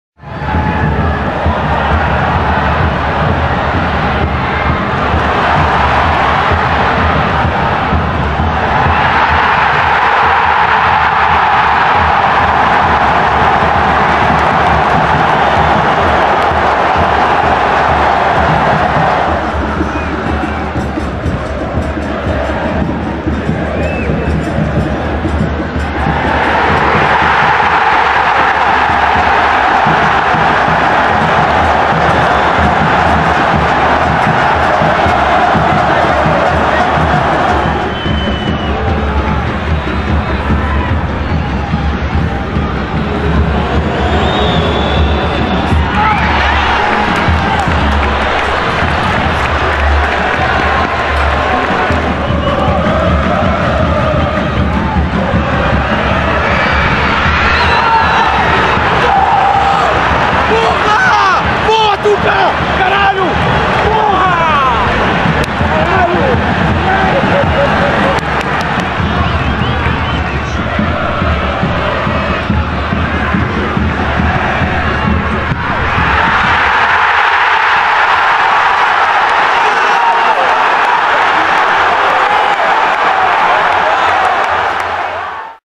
VEJA OS GOLS - Fluminense 3 x 2 Figueirense - Brasileirão 2016
Campeonato Brasileiro de Futebol:
Serie A

Resultado:
Fluminense 3 x 2 Figueirense

Dia do Jogo:
Sábado, 3 de Setembro

Resumo:
Em uma partida bastante movimentada no Giulite Coutinho, o Fluminense venceu o Figueirense por 3 a 2, neste sábado, em partida atrasada, que valeu pela 18ª rodada do Campeonato Brasileiro.